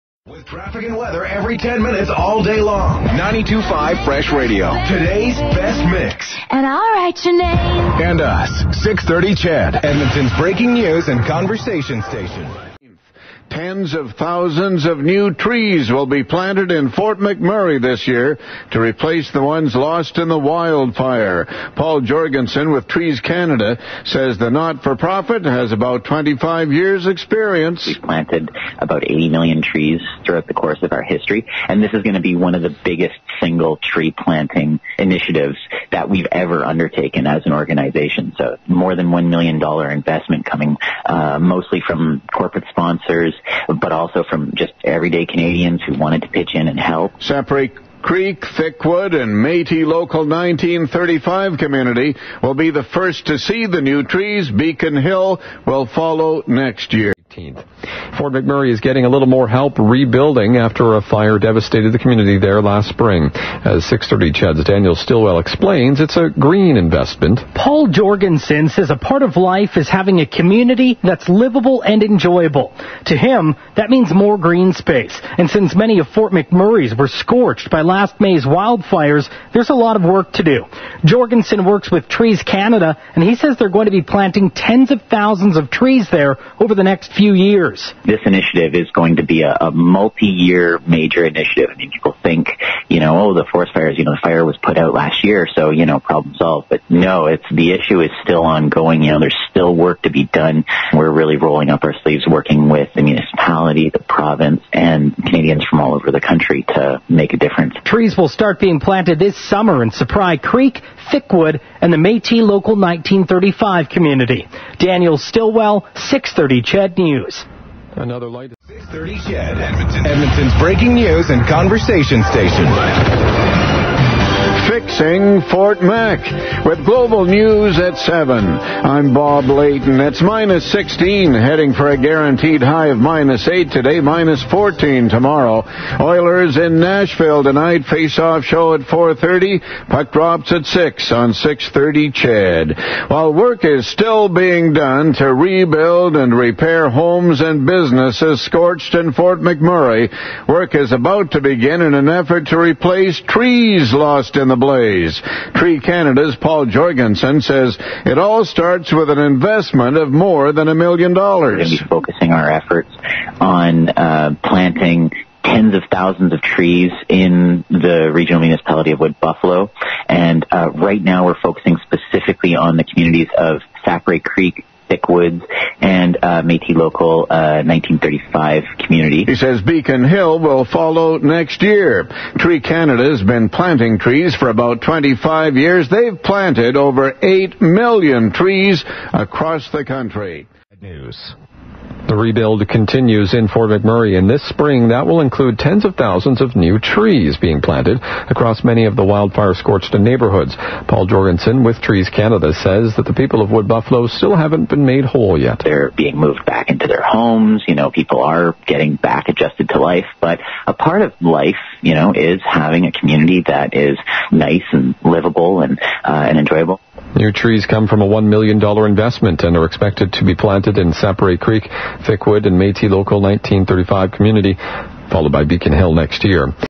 Tree Canada will be planting trees in Fort McMurray - CHED 630 radio
Tree Canada announces more than $1 million in funding for the Operation ReLeaf - Fort McMurray reforestation project starting in spring 2017.